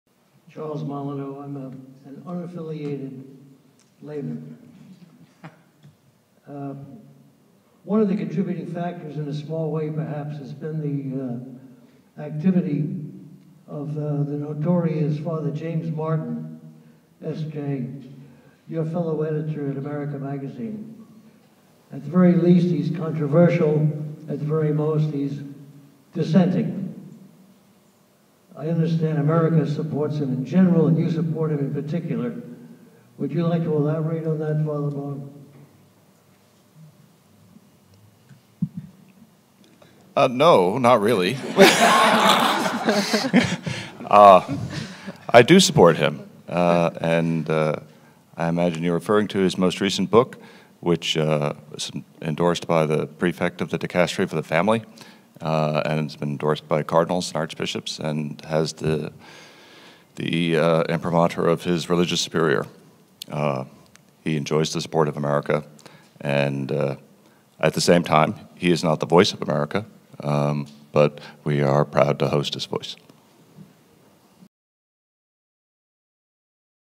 Audience question for Malone re: James Martin, SJ
In this clip, an audience member asks Fr. Matt Malone, SJ, editor of America Magazine, about his support for Fr. James Martin, SJ. From the session, "How do we reflect these causes in narratives in the press?" at "What's Really Going On? The Root Causes of the Current Crisis" from the Healing the Breach of Trust series at the Catholic University of America, with the Catholic Project.